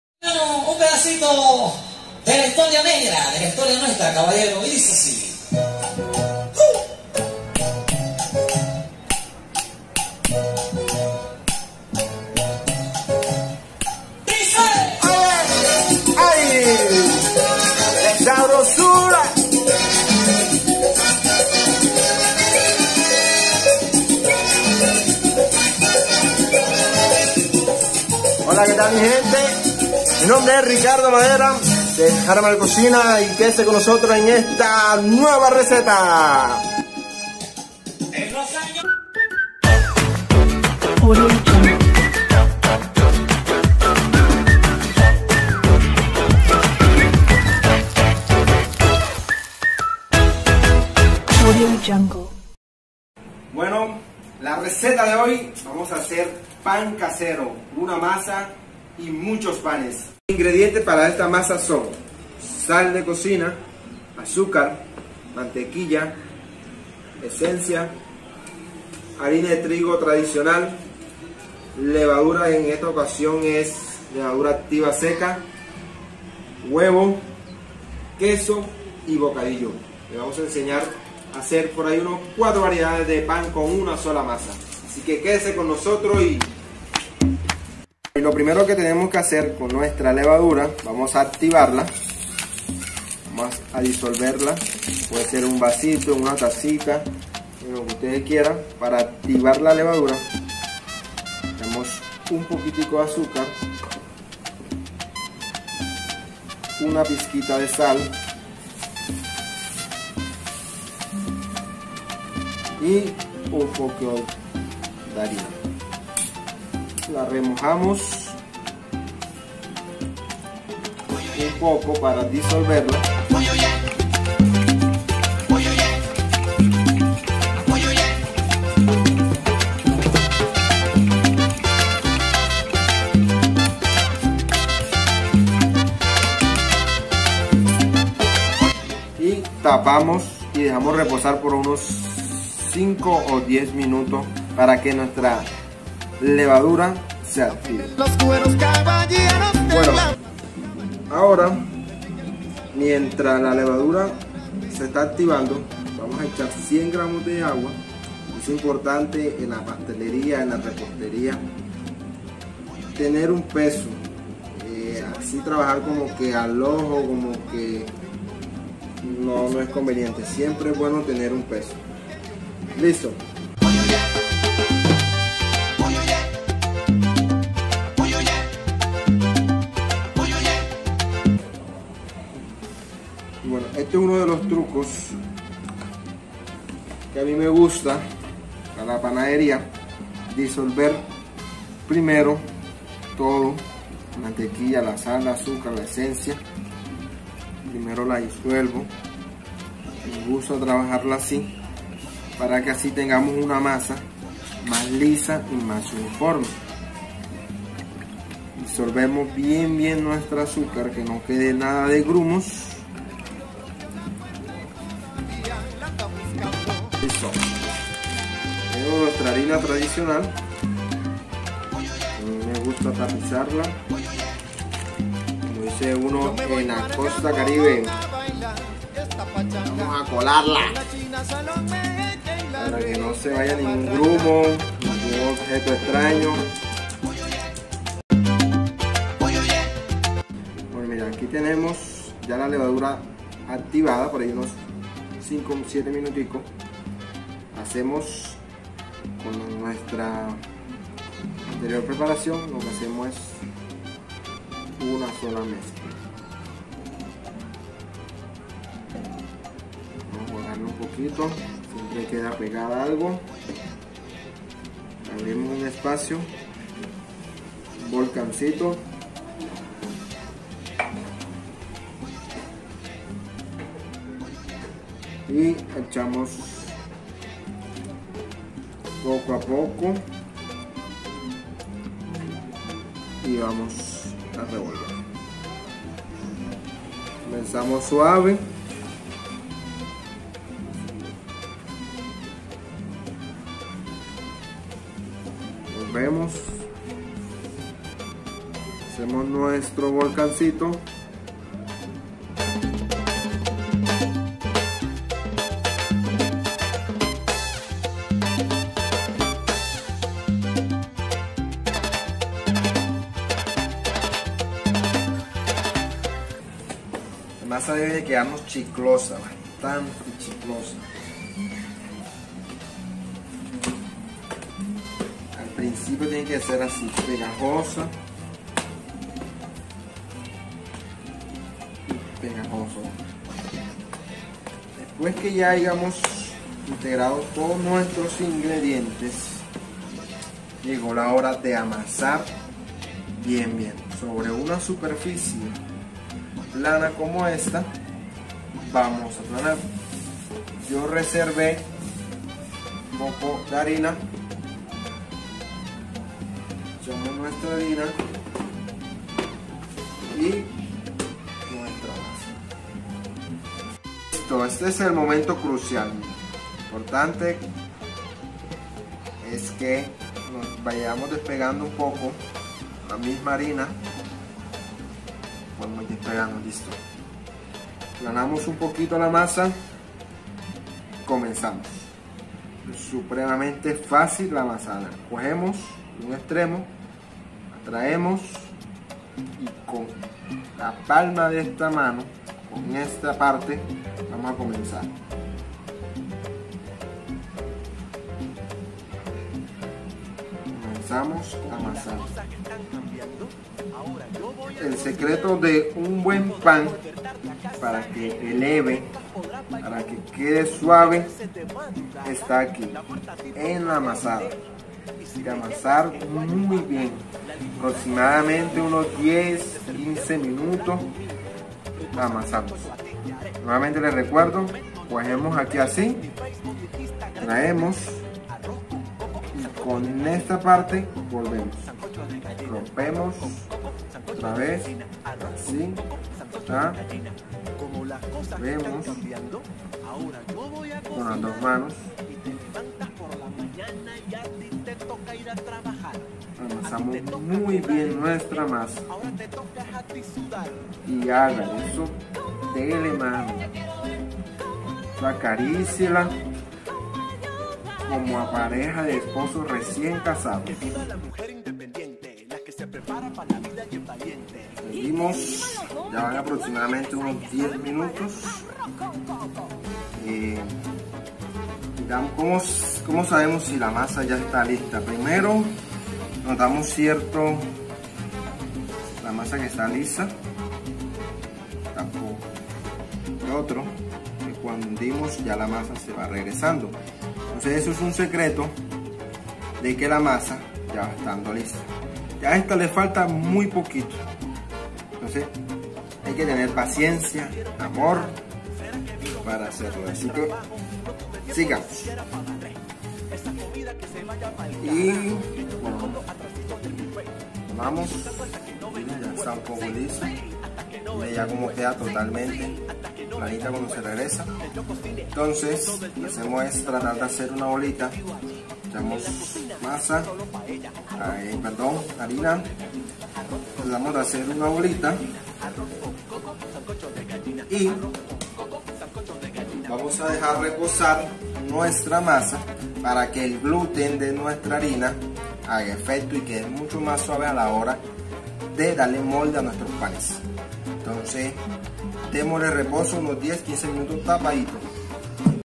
pan casero parte 1 (JARMAL COCINA , hecho con amor para toda ocasión)
# masa básica para pan
pan agridulce
pan de queso y bocadillo
pan trenza
rosquete de bocadillo
# pan hecho en casa